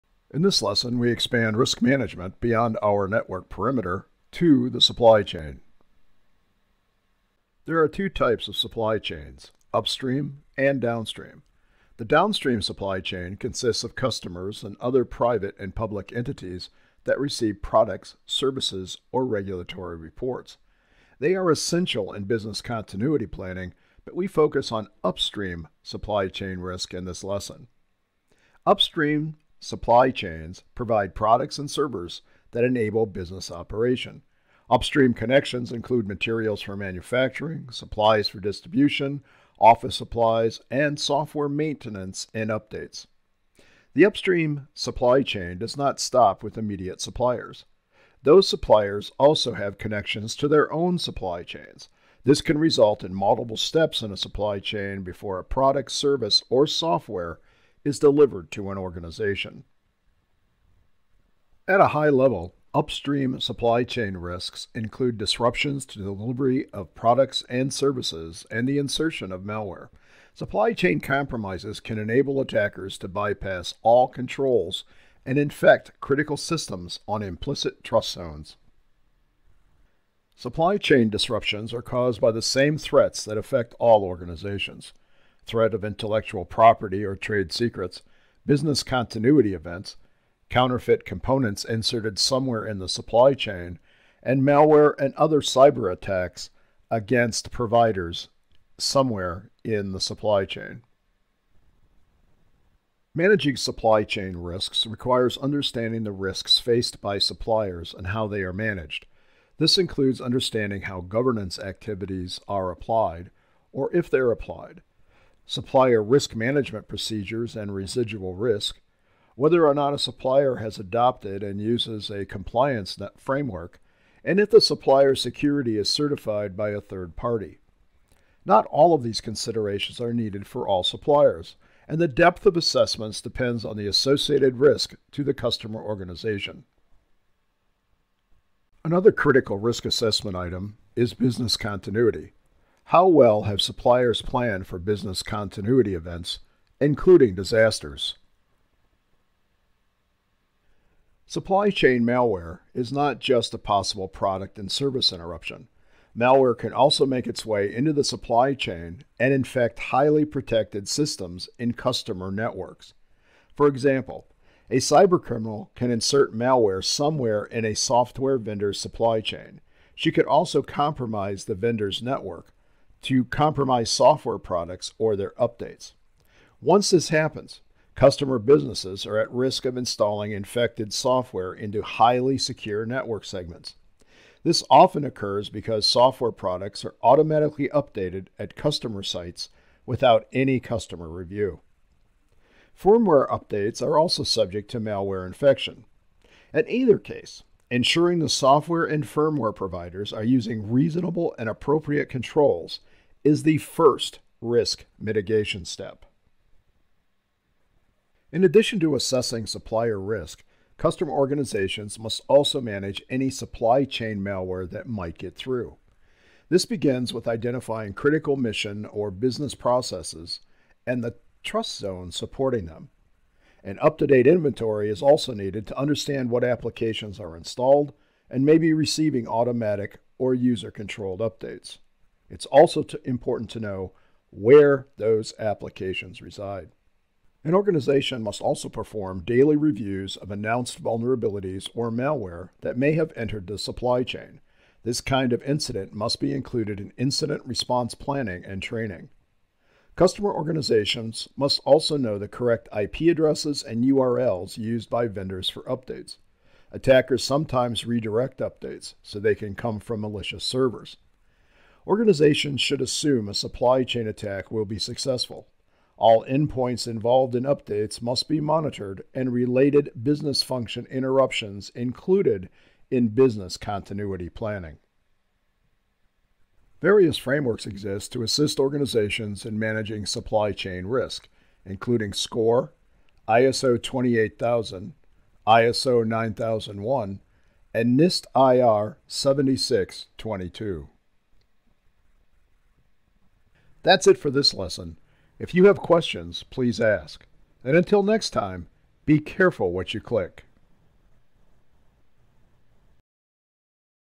Supply Chain Risk Management
In this lesson, we look at the security risks associated with the upstream supply chain, including

00:00 - Intro
00:07 - Definition of supply chain
01:01 - Supply chain risks
01:20 - Causes of supply chain disruptions
01:43 - Managing general supply chain risk
02:31 - Supply chain malware infection
03:28 - Managing supply chain malware compromises
04:36 - Supply chain risk management  frameworks